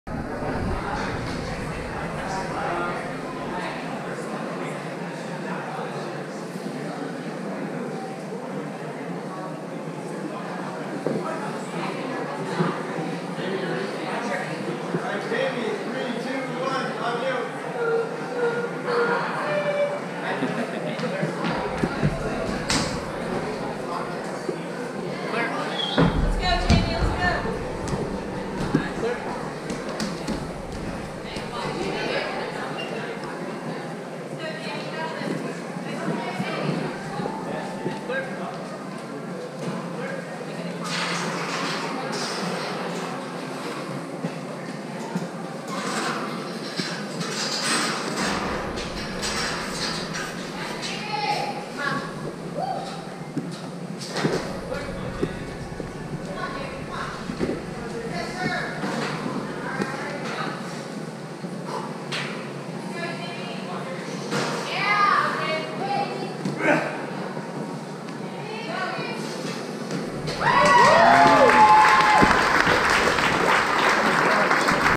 Jamie Rahn 3rd Place Obstacle Academy | National Ninja League Season 5
A former podium finisher at the World Championship, "Captain NBC" Jamie Rahn gets another podium at Obstacle Academy and punches his ticket to Greensboro.